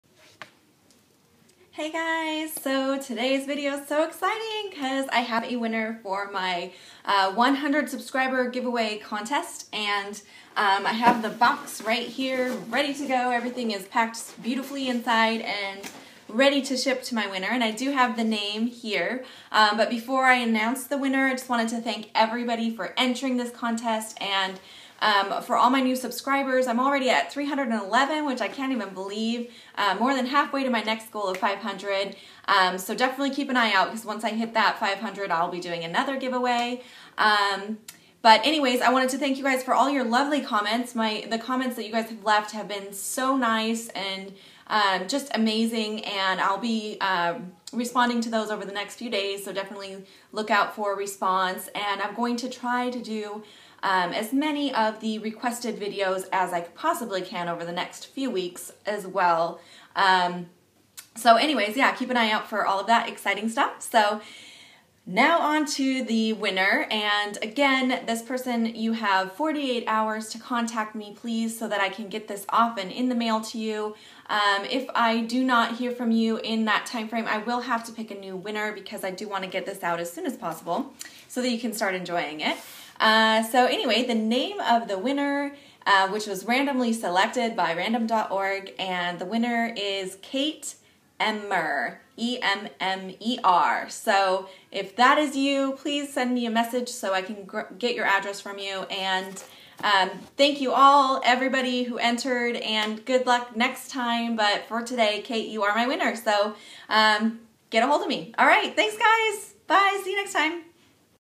100 subscriber giveaway winner announcement!
Kate Emmer you are the winner! Congrats to you!

Shop with me:
link to sign up for haute look:

The camera I use to film:
Samsung HMX-F90